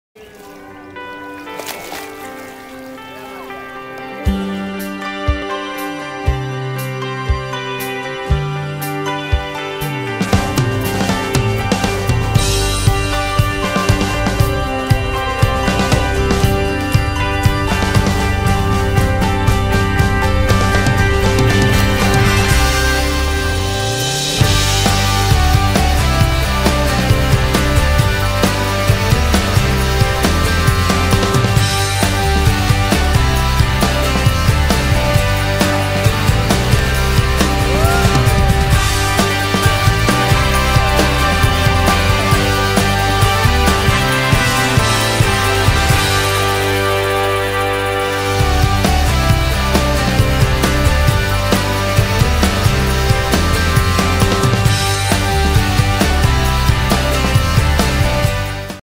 Boia Infantil de Braço Salva Vidas - Momis Petit
A Boia colete e braço proporciona segurança na hora de brincar na água. Ela possui fecho nas costas e é homologada pela Marinha brasileira.